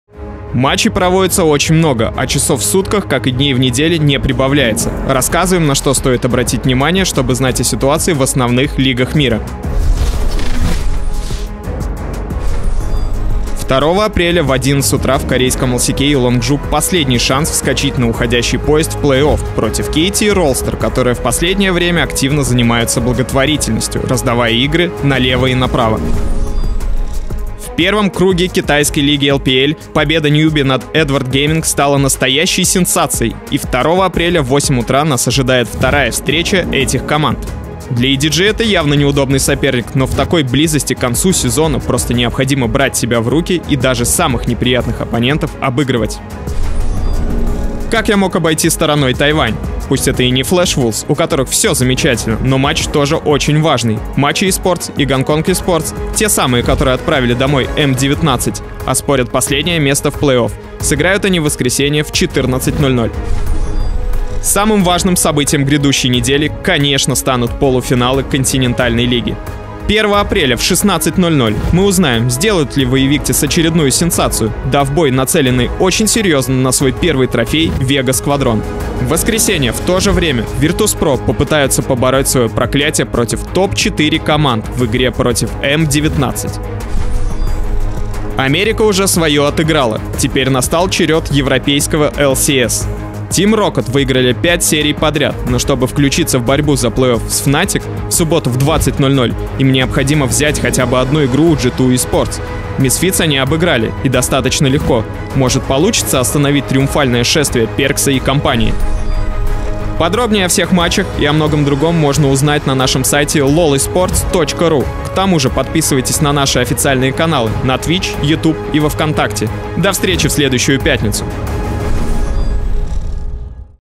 Что посмотреть? 1-7 апреля
ATRemains подскажет, что вам нужно посмотреть на следующей неделе, чтобы не пропустить важные матчи!

Все новости Континентальной лиги (LCL), включая расписание, результаты матчей и др., доступны на САЙТЕ: